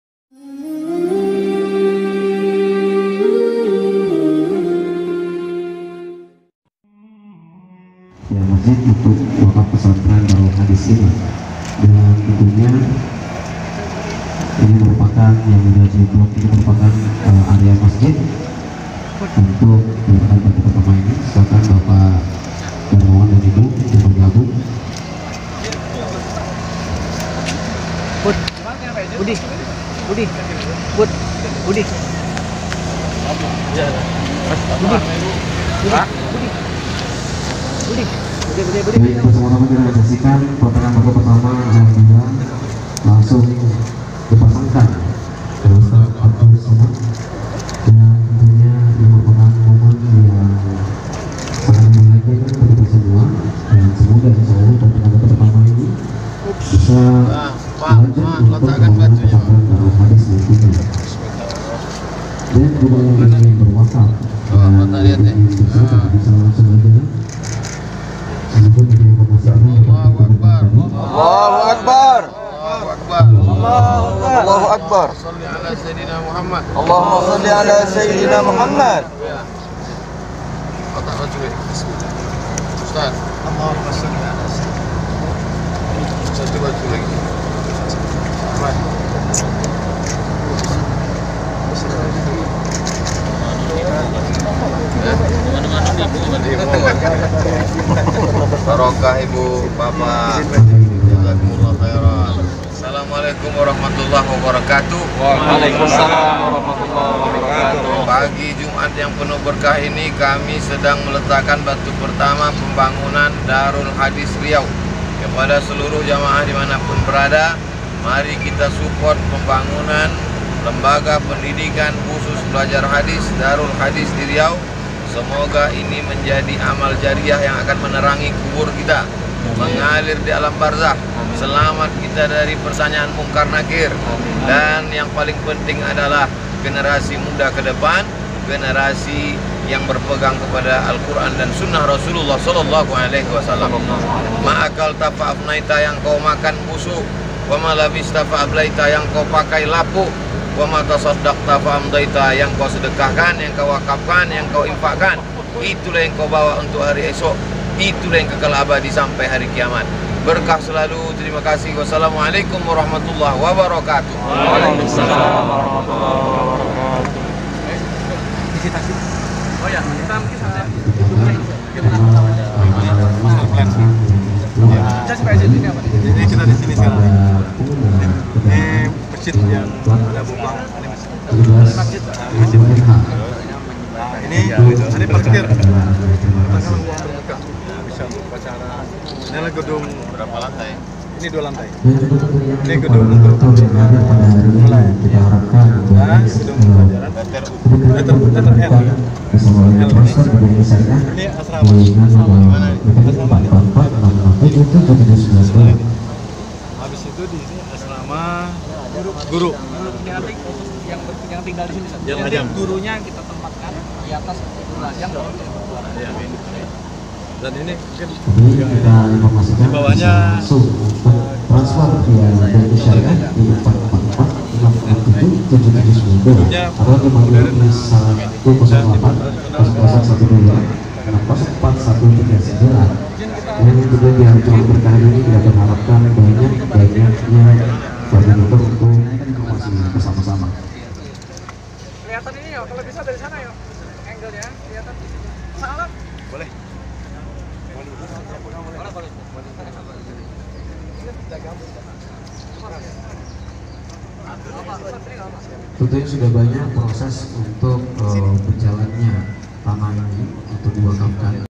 Ustadz Abdul Somad Meletakkan Batu Pertama Pesantren Darul Hadist Pekanbaru
Peletakkan Batu Pertama Pembangunan Pesantren Mahasiswa Takhasus Hadis Darul Hadis Riau Dompet Dhuafa Riau dan bedah konsep Darul Hadis bersama UAS.

Wakaf lahan seluas 55 x 35 m yang awalnya diamanahkan kepada UAS, lalu oleh UAS diarahkan ke Dompet Dhuafa Riau, hari ini Alhamdulillah sudah down breaking bersama UAS, Asatidz Cordofa Riau, para donatur dan masyarakat.

Pesantren ini berada di Km 1 jl. Garuda Sakti Panam Pekanbaru. Sekitar 200 m dari perempatan Panam. Insya Allah akan dibangun kolaborasi Dompet Dhuafa Riau dan Yayasan Tabung Wakaf Umat. Pesantren ini berbeda dengan Pesantren Nurul Azhar Palas yang dibangun oleh Yayasan Tabung Wakaf Umat.
Semoga Allah mudahkan dan pusat pengkajian Ahlussunnah wal Jama'ah semakin subur di Tanah Melayu. Amin.